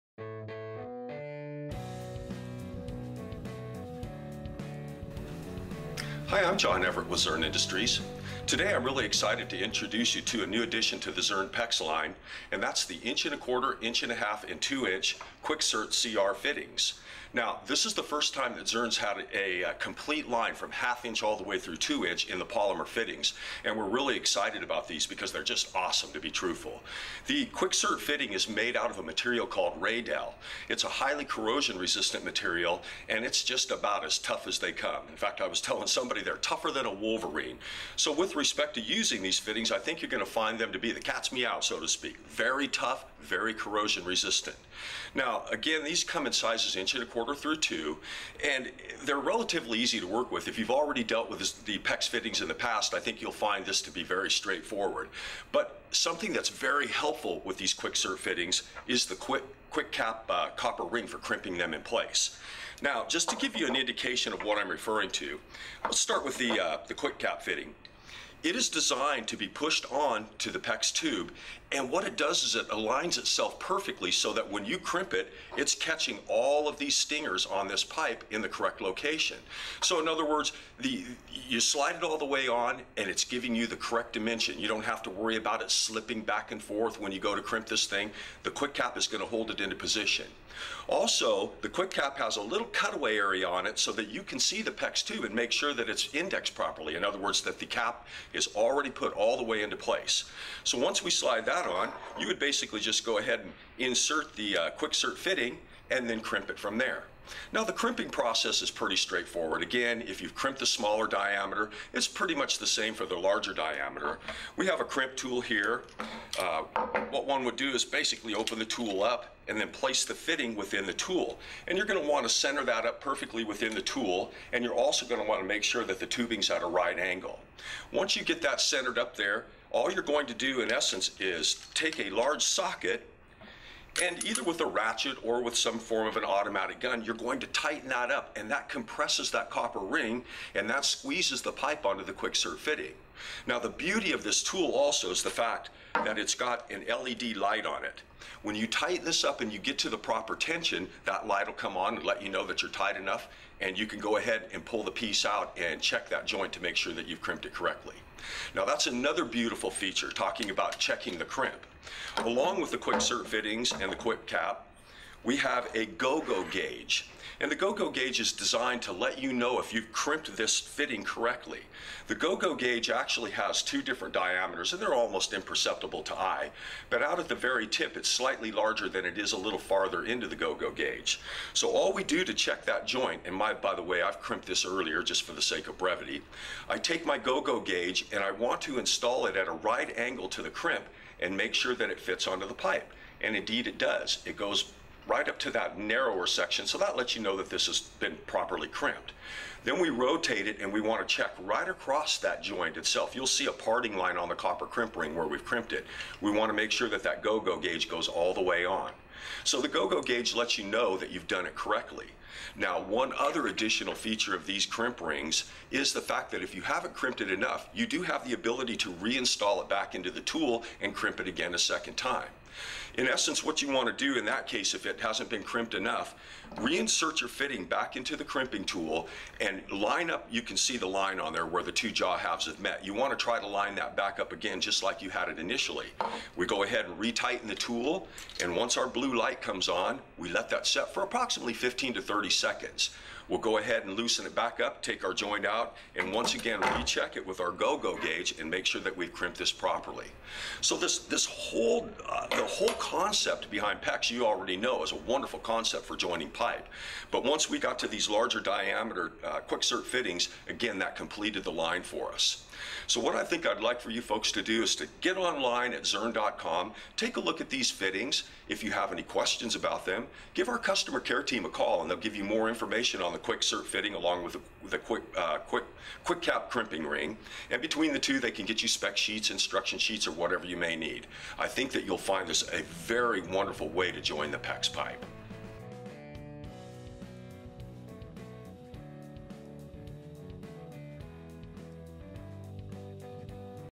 Zurn PEX Plumbing Large Diameter Qick Sert CR® Polymer Fittings
Zurn is proud to introduce our new large diameter Qick Sert CR polymer couplings, elbows, and tees, providing a complete labor-saving polymer fitting solution from 1/2" - 2". Qick Sert CR fittings are durable and corrosion resistant, ideal for aggressive water environments.